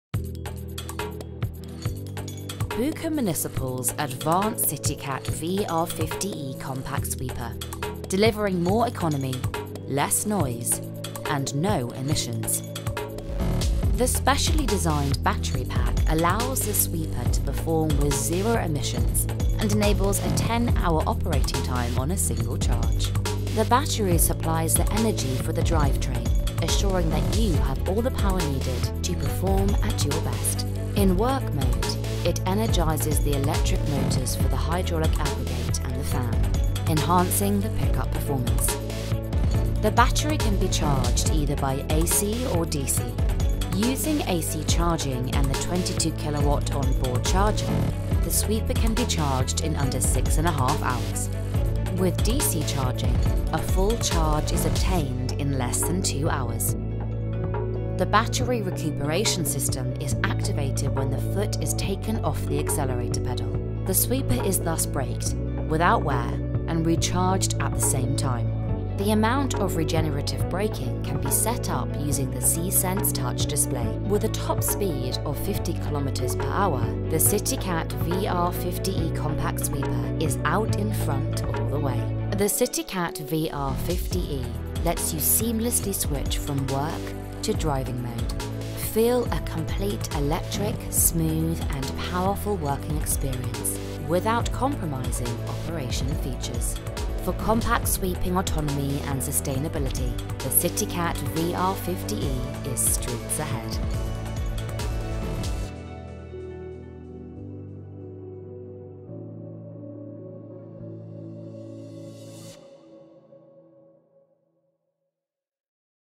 Bucher CityCat VR50e - Electric Drive Train
The new Bucher CityCat VR50e, our all-electric high performer in the compact class. The Bucher Battery Pack with a capacity of 137 kWh enables continuous sweeping operation for up to ten hours after only 1.5 - 2 hours of charging time.